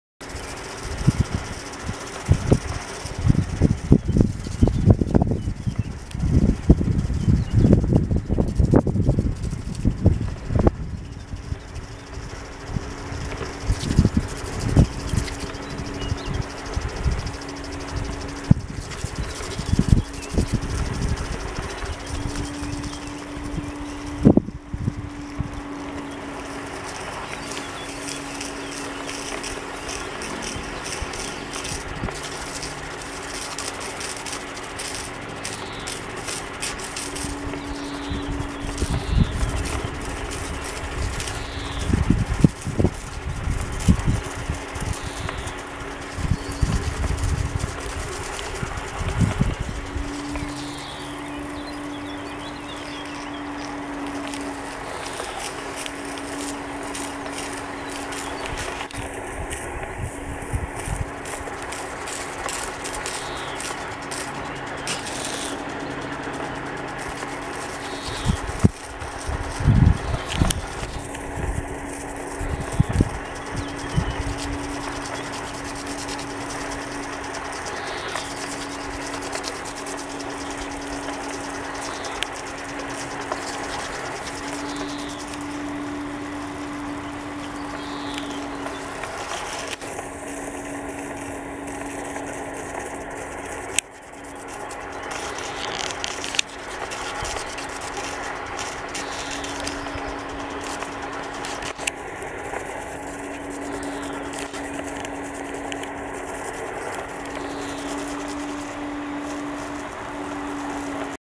Boxy's erster Wasserspaß
Endlich floss das Wasser wieder- Boxy war überglücklich. Mai 2016